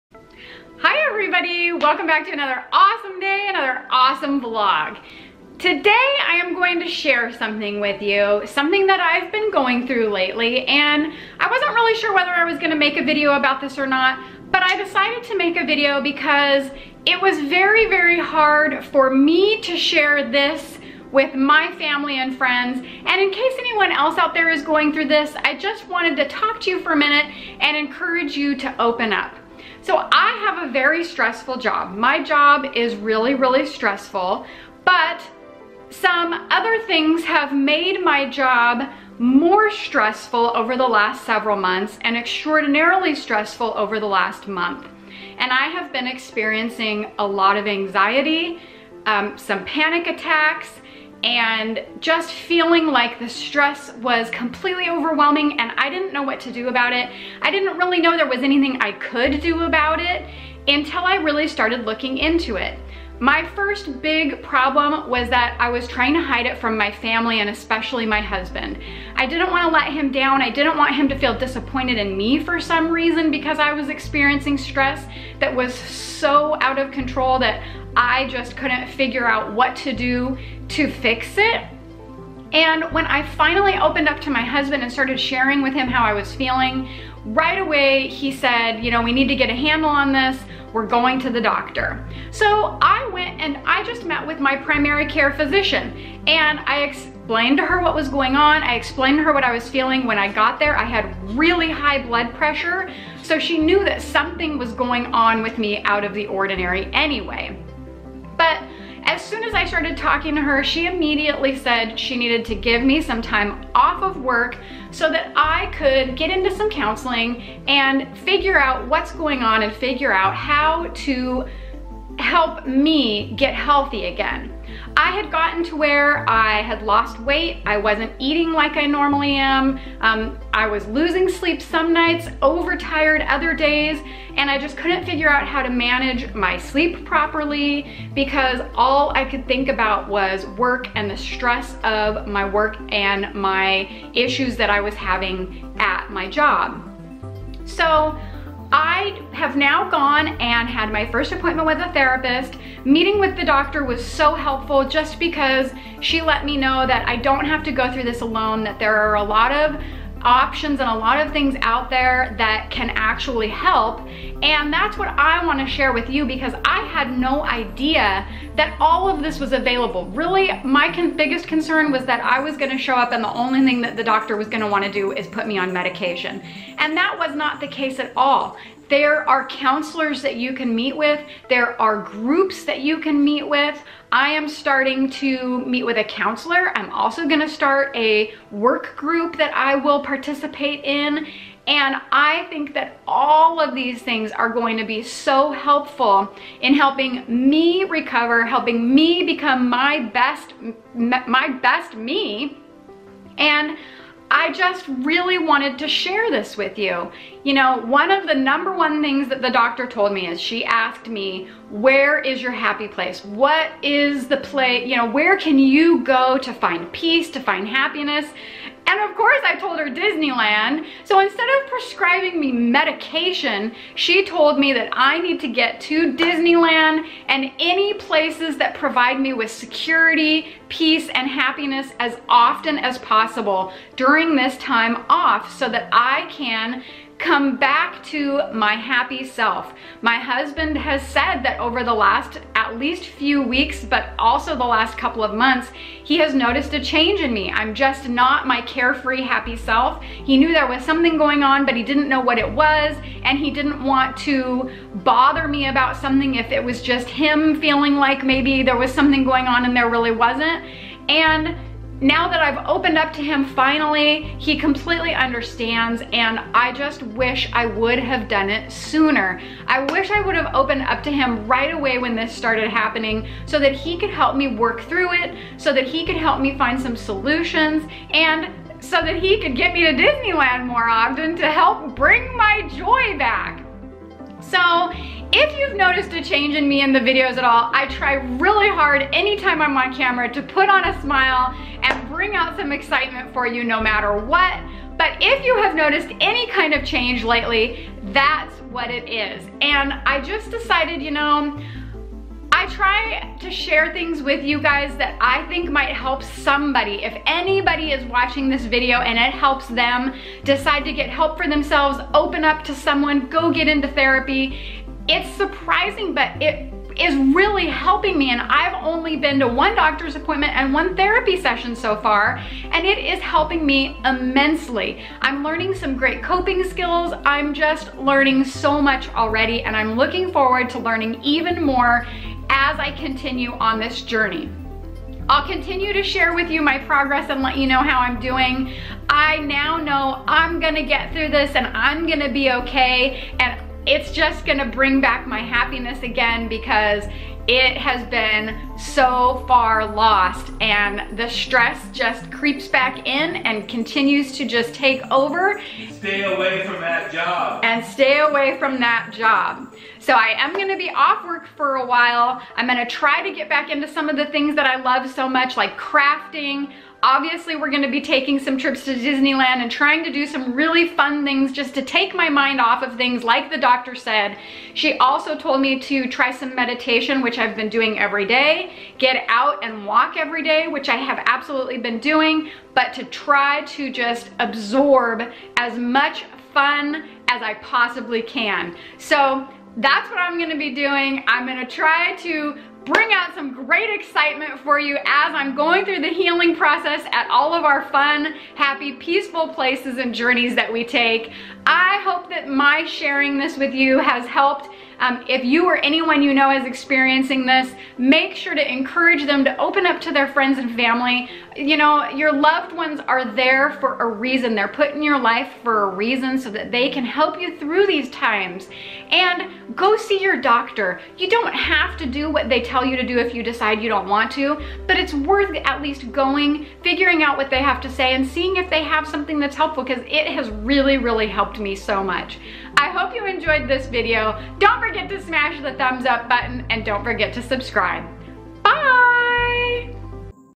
I Have Been Living a Lie and Not Being Truthful About Something. Not Anymore. I Open Up Here.
I decided to open up and share something with you that I have been hiding from everyone.  This video will explain everything and hopefully help someone else to open up and get the help they need.  Have an awesome day, Billisa.